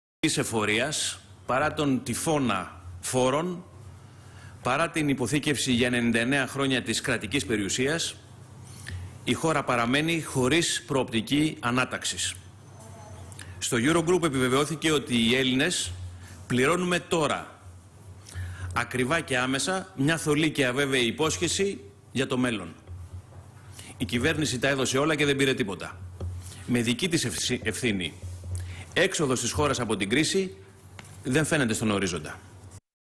Ν.Δ.: Προπαγάνδα τεχνητής ευφορίας από την κυβέρνηση
«Η χώρα με ευθύνη της κυβέρνησης είναι χωρίς προοπτική ανάταξης», δήλωσε ο εκπρόσωπος Τύπου της Ν.Δ. Γιώργος Κουμουτσάκος σχετικά με την απόφαση του Eurogroup.